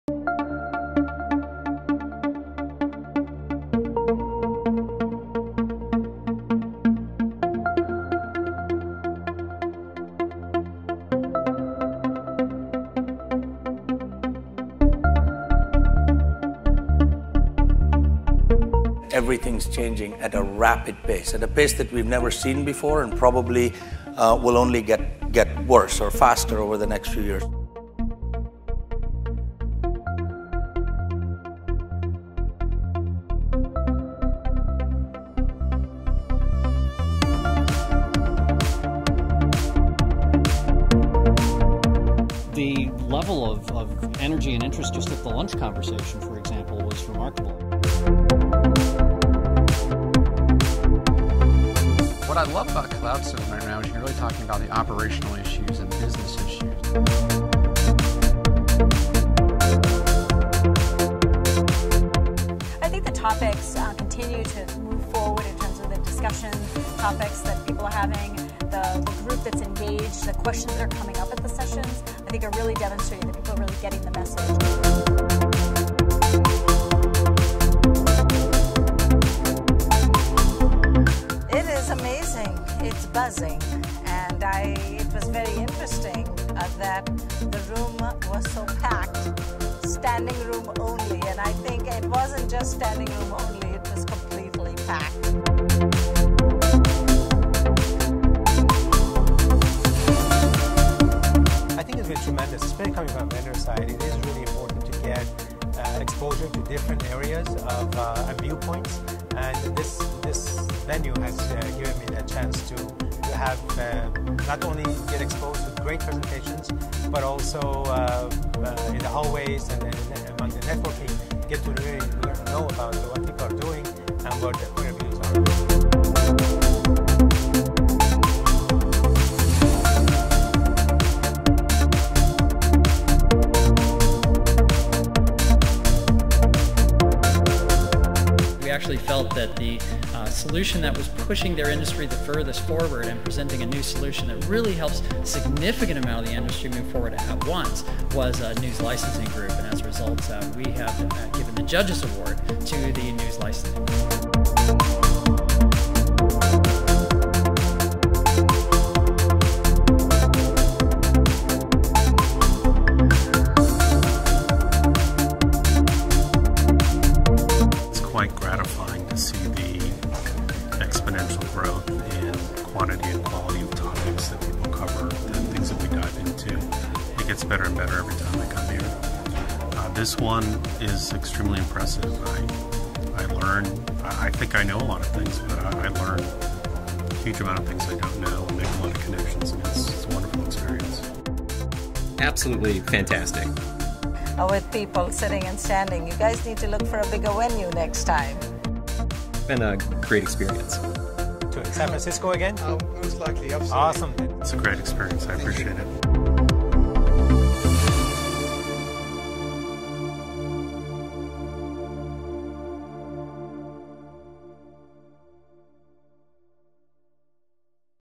UP 2014 HIGHLIGHTS
UP conference is uniformly regarded as an essential resource for businessmen, scientists, engineers, researchers, developers, and IT managers, who come for the world-class education, the unrivalled peer-to-peer collaboration, and exposure to the most innovative products and solutions on the market today.